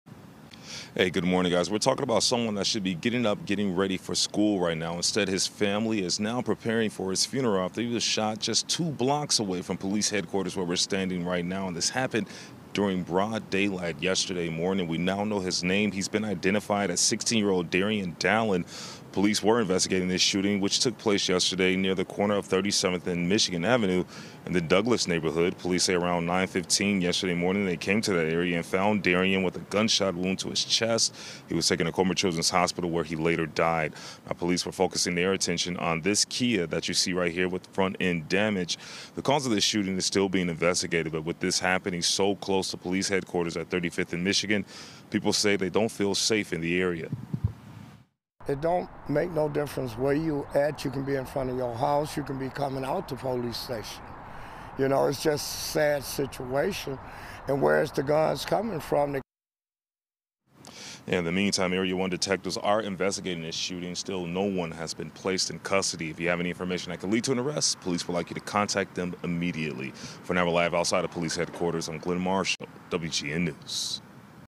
Boy, 16, fatally shot in the chest on Chicago's South Side
A 16-year-old boy is dead after he was shot in the chest on the South Side of Chicago, according to police.